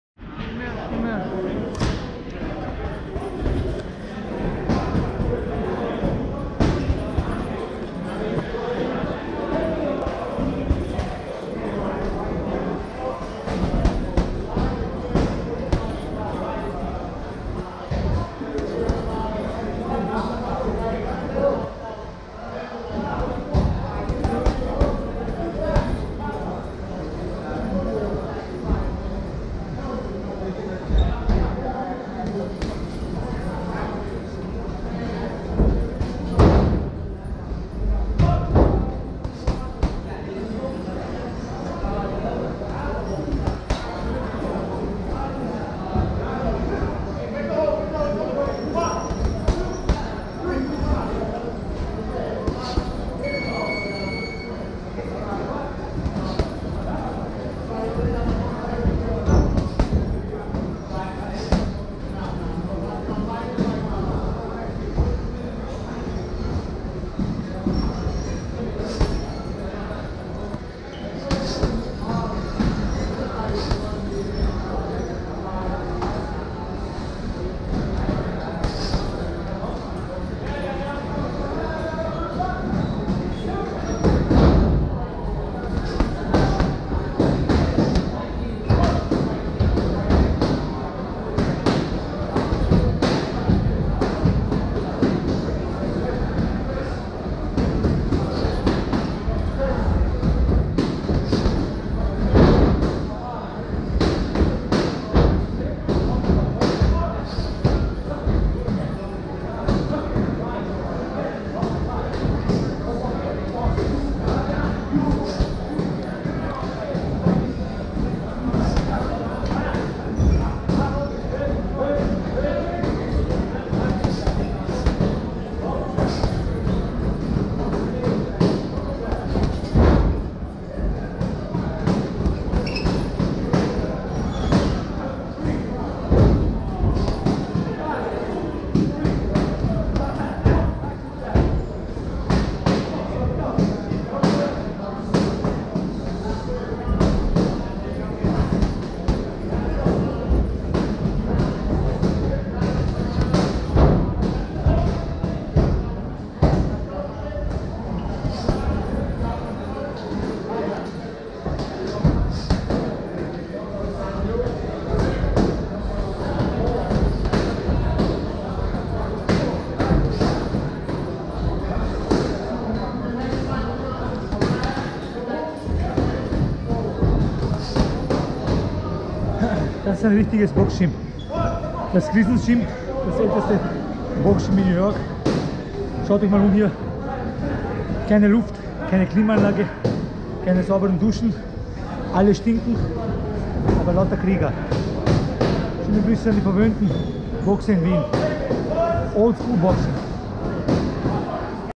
WBC East Europe Champ Gogi Knezevic at Gleasons Gym Brooklyn New York
Gogi Knezevic four days up to Edgar Berlanga Fight in Queens New York